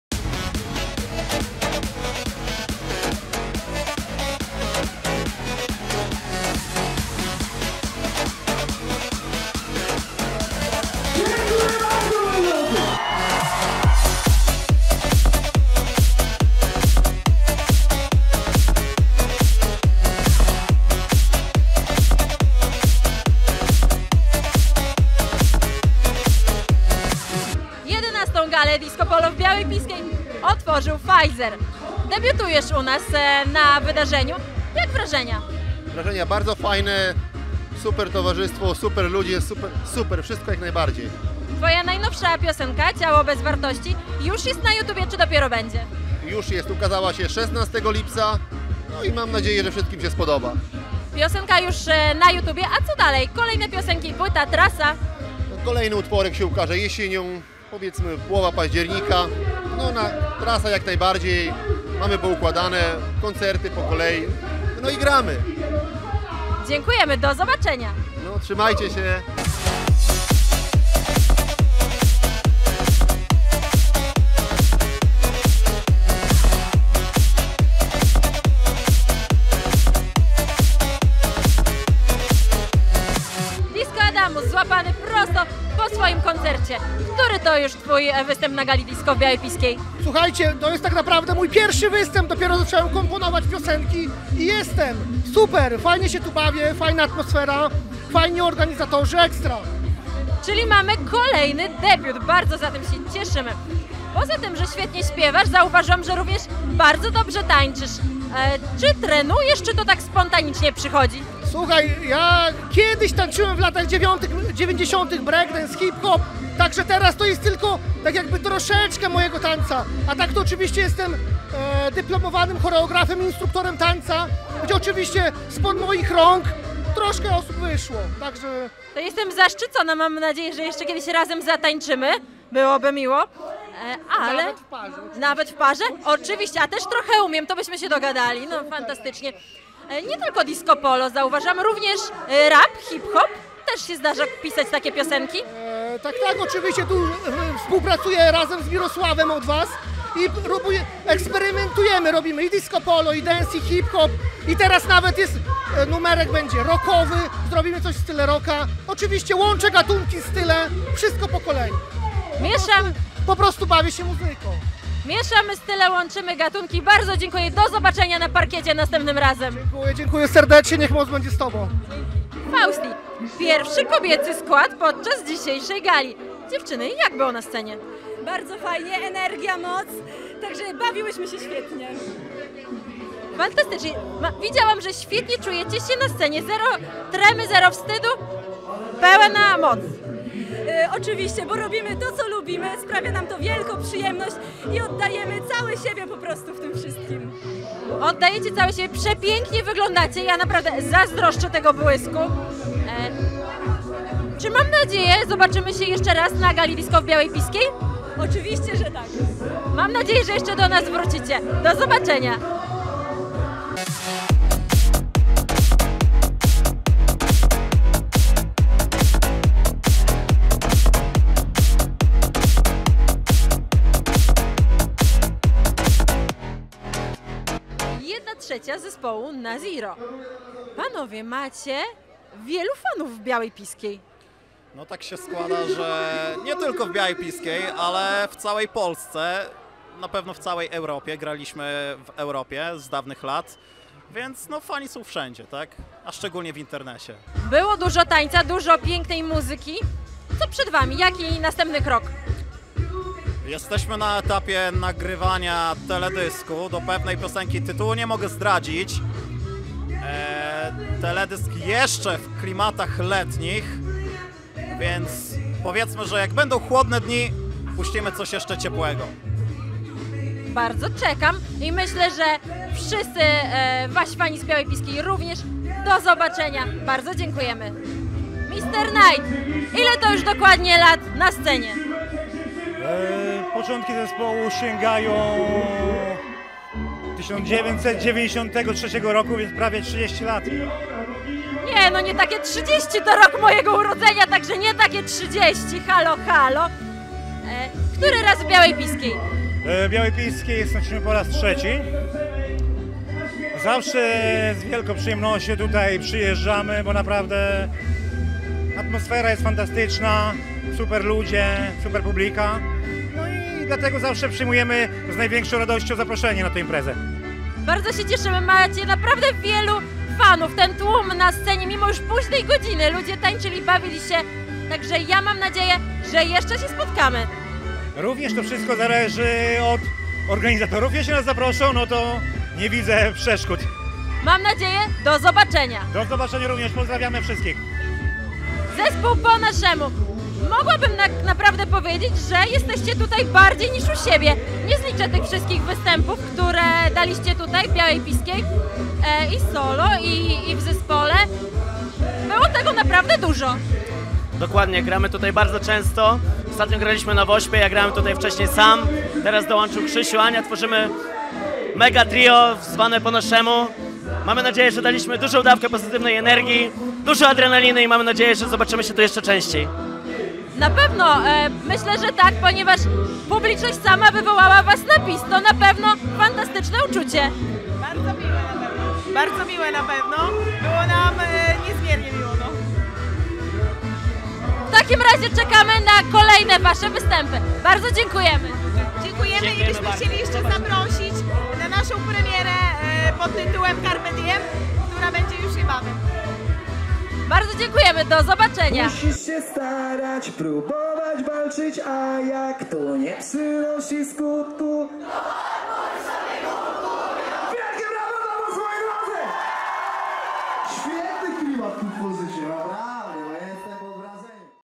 Relacja z XI Gali Disco w Białej Piskiej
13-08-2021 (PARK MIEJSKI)

►Realizacja wideo : M-GOK TV
Dominika / Miroslav

►Wielkie podziękowania dla :
    Kamila Świderskiego (FOTO KAMIL)
    oraz Piotra Strzyżewskiego

    za pomoc przy realizacji.